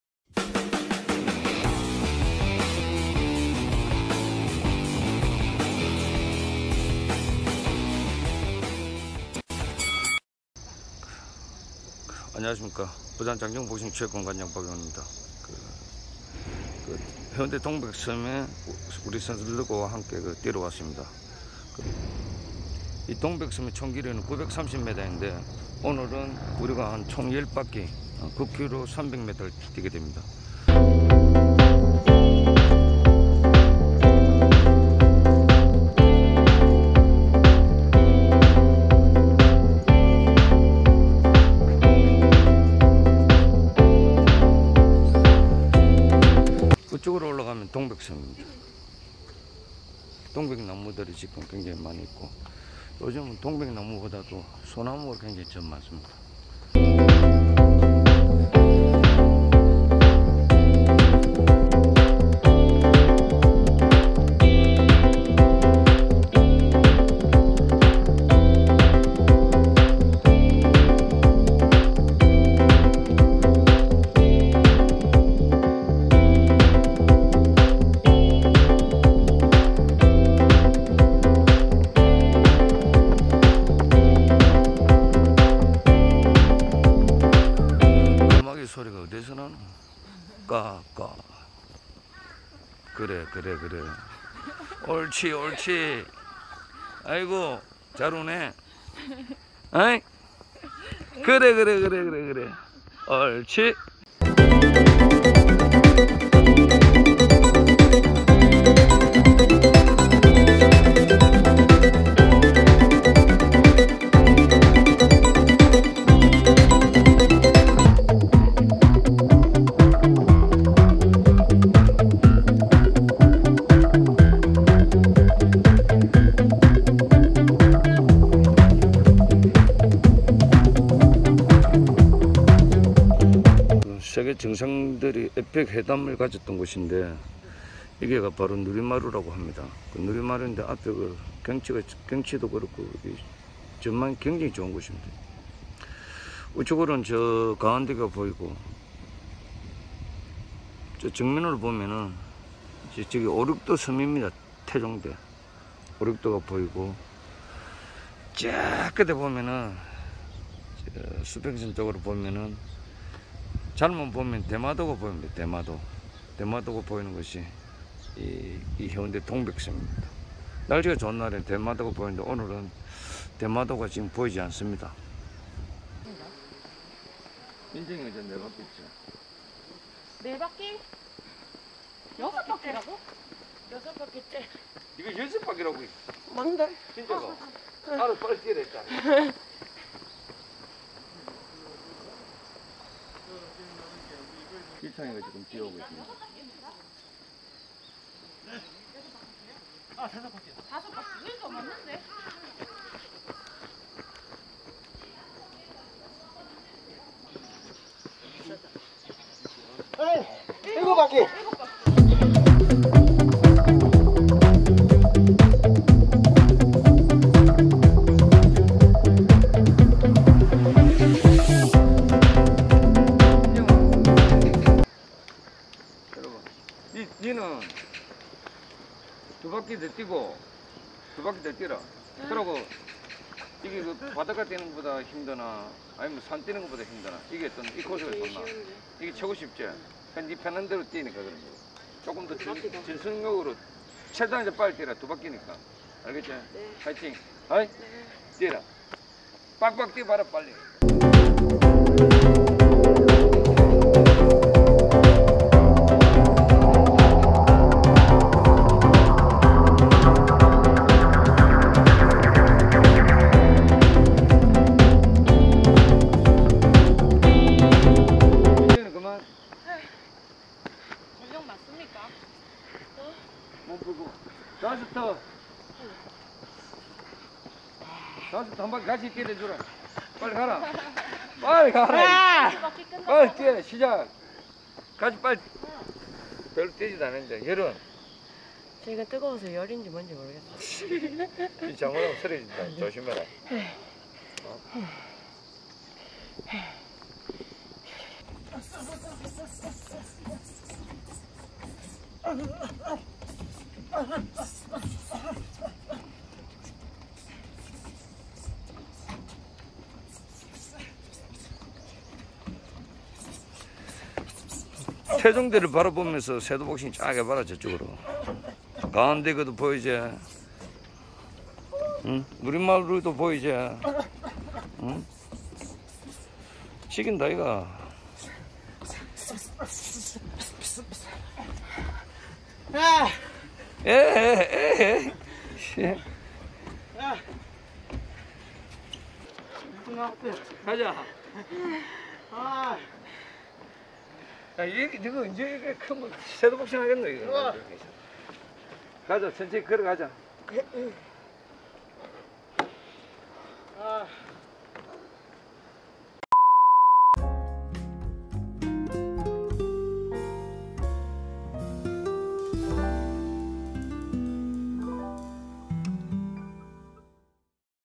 프로 복싱선수들은 아침운동 어떻게 할까, 어디서 할까?! 해운대 동백섬!!
프로 복싱선수들은 아침운동 어떻게 할까 ?! 어디서 할까?! 해운대 동백섬편 !!!